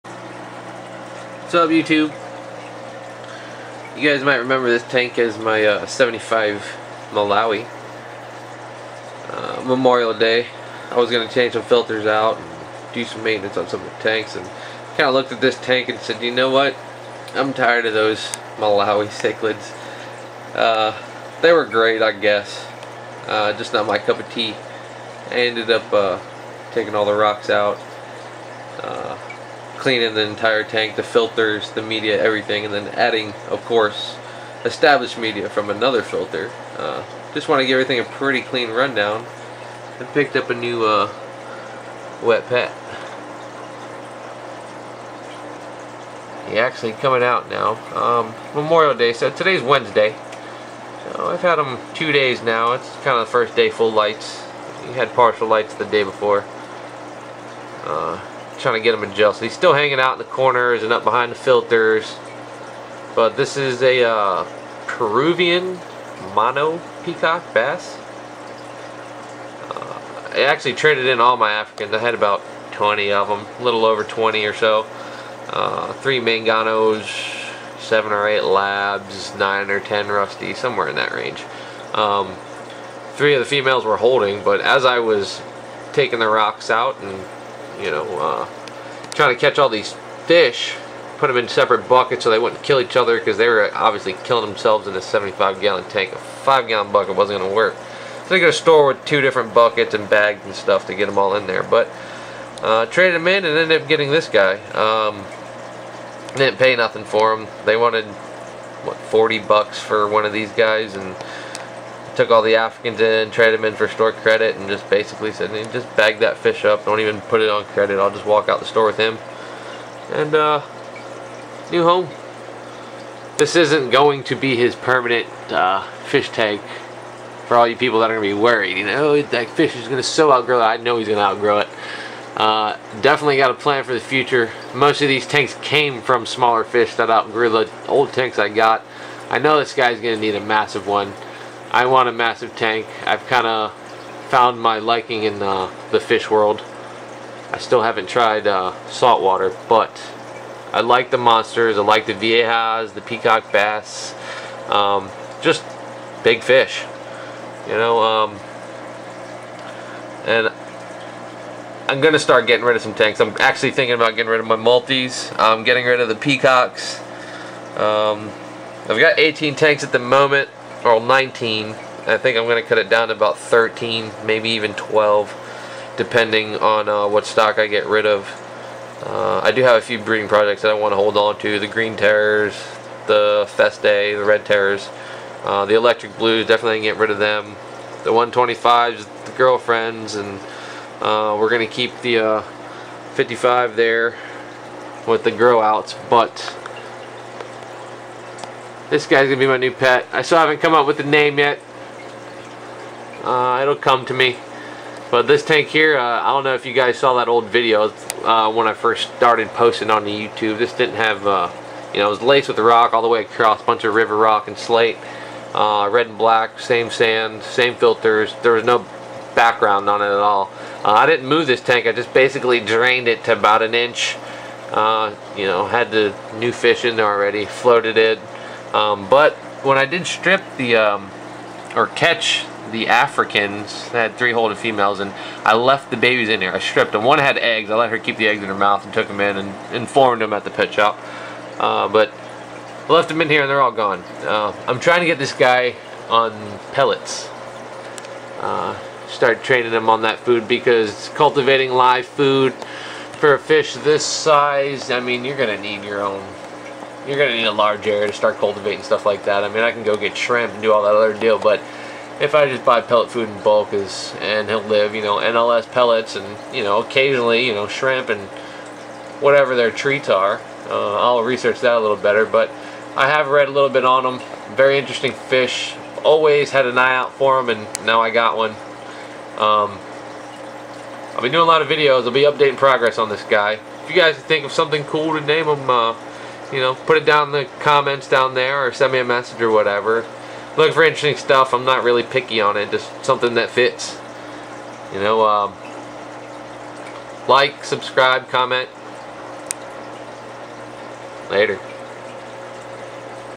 The Switcharoo Week 1 Pbass
Swapped out stock and overhauled my old Mbuna tank. Here is the new wet pet.